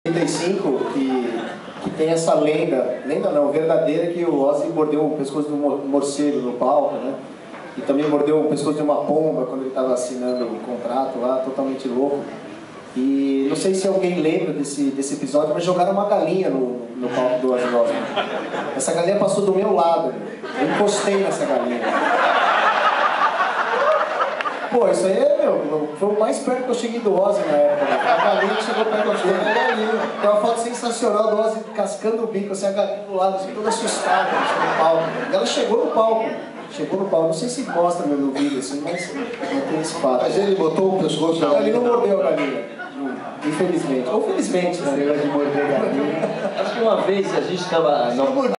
andreas kisser @ teatro rival: falando sobre o show de ozzy osbourne no rock in rio
19/09/11 - "encontros o globo - rock in rio": debate com andreas kisser, tony bellotto, branco mello, dado villa-lobos e léo jaime - mediação arthur dapieve & bernardo araújo / teatro rival (cinelândia / rio de janeiro)